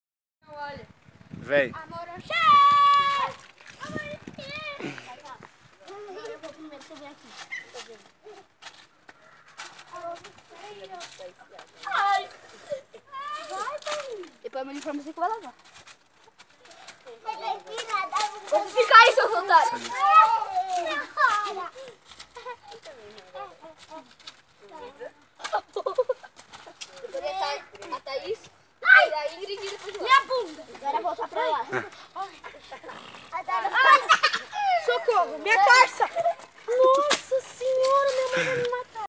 Sobre Domingo ! A alegria com meus alunos e família(2)
Jardim Botânico em Jundiaí -SP